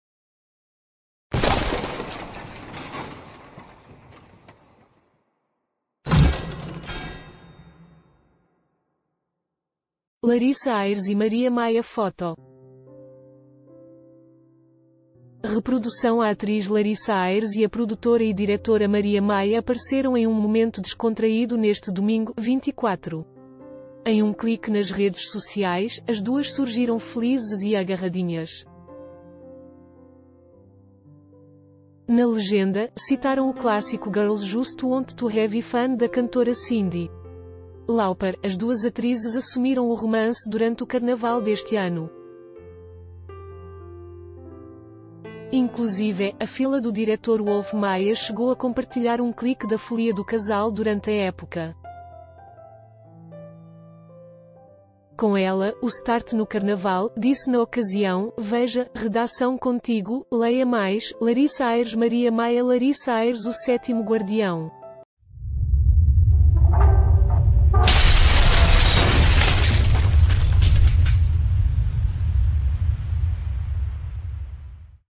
Maria Maya e Laryssa Ayres surgem em momento descontraído nas redes sociais
Laryssa Ayres e Maria Maya Foto. Reprodução A atriz Laryssa Ayres e a produtora e diretora Maria May...